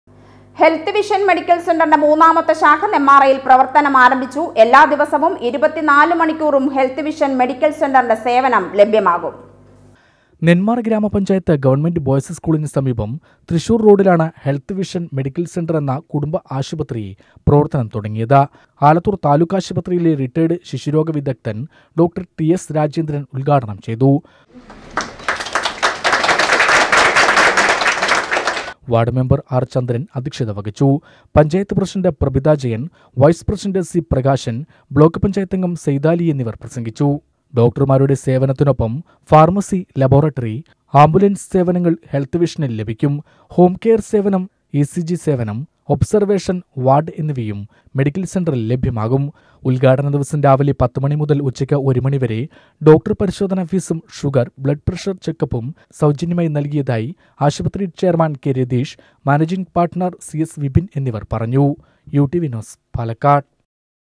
ഹെൽത്ത് വിഷൻ മെഡിക്കൽ സെന്ററിന്റെ മൂന്നാമത്തെ ശാഖ നെന്മാറയിൽ പ്രവർത്തനം ആരംഭിച്ചു.
ഹെൽത്ത് വിഷൻ മെഡിക്കൽ സെന്ററിന്റെ മൂന്നാമത്തെ ശാഖ നെന്മാറയിൽ പ്രവർത്തനം ആരംഭിച്ചു. എല്ലാ ദിവസവും 24 മണിക്കൂറും ഹെൽത്ത് വിഷൻ മെഡിക്കൽ സെന്ററിന്റെ സേവനം ലഭ്യമാക്കും.